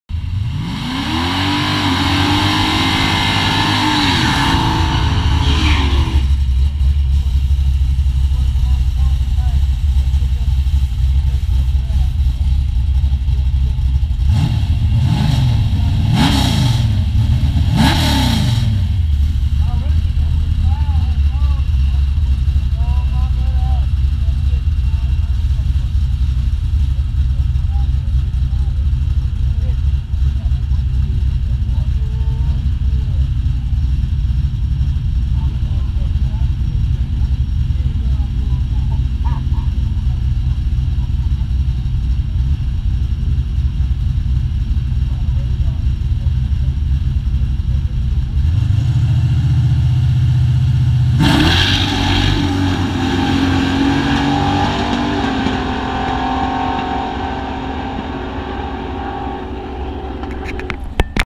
Two nice GM Trucks Racing, Fast
Two nice trucks drag racing at Texas Raceway, Kennedale. Onr Chevy and one GMC